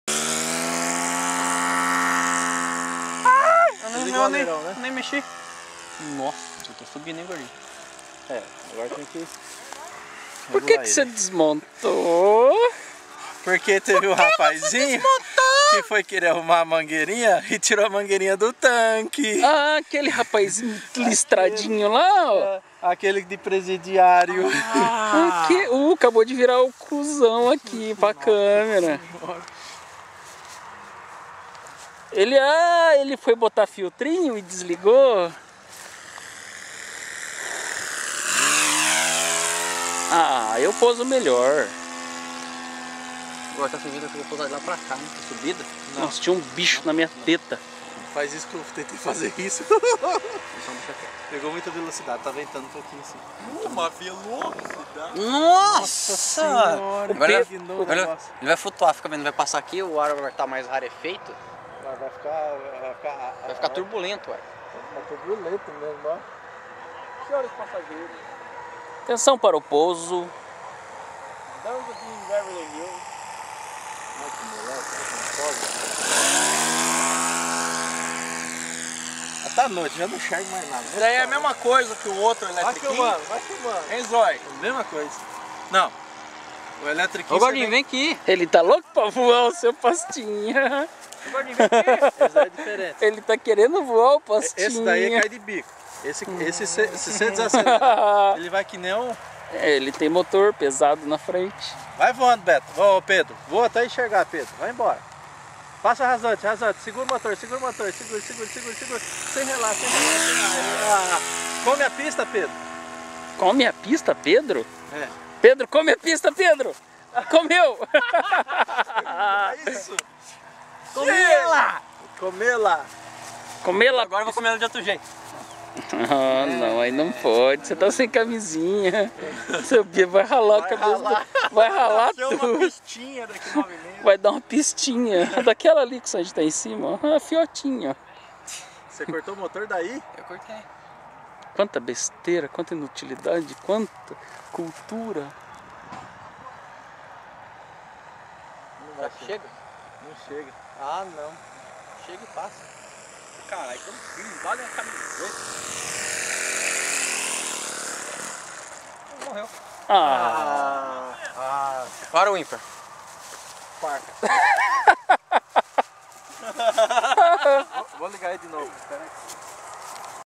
Pedro voando e comendo pista de Sta Cruz do Rio Pardo SP
Pedro comendo pista Sta Cruz do Rio Pardo SP